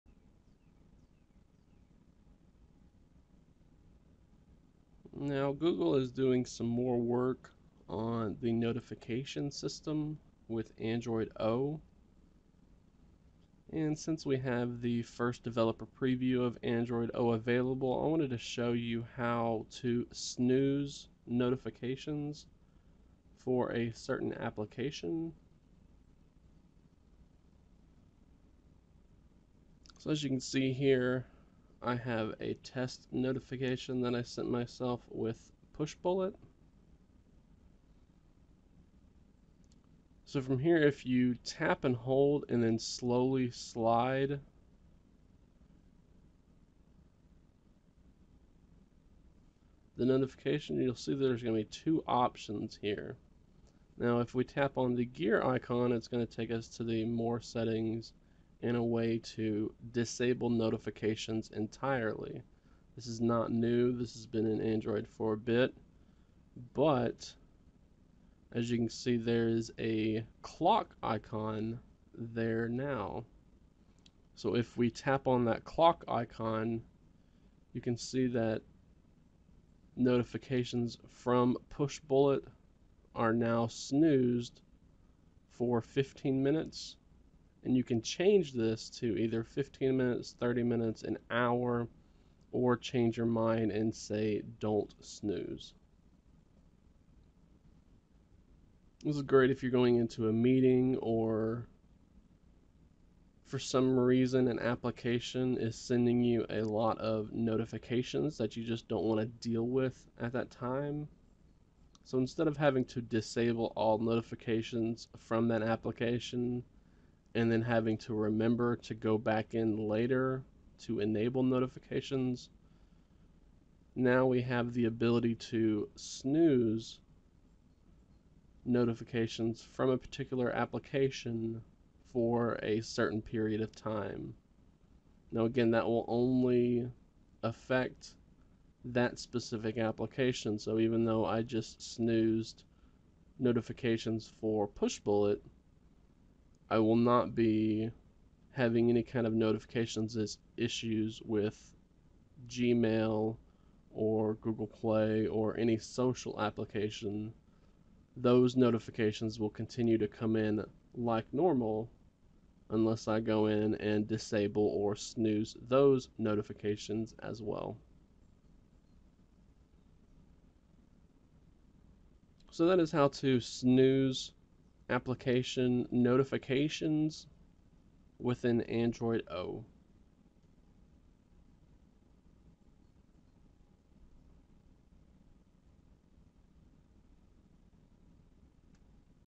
[Android O] Snoozing Notifications
With DP1, we now have the ability to ignore notifications for a certain amount of time with the Snooze Notifications feature that is baked into Android O.